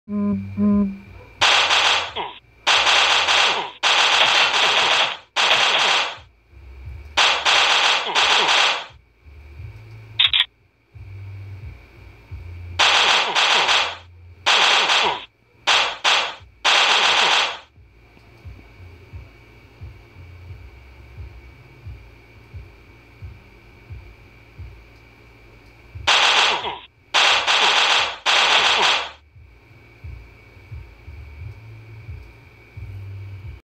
A game made by me
This game was made using fps maker by Silentworks! gun textures,map design is made by me.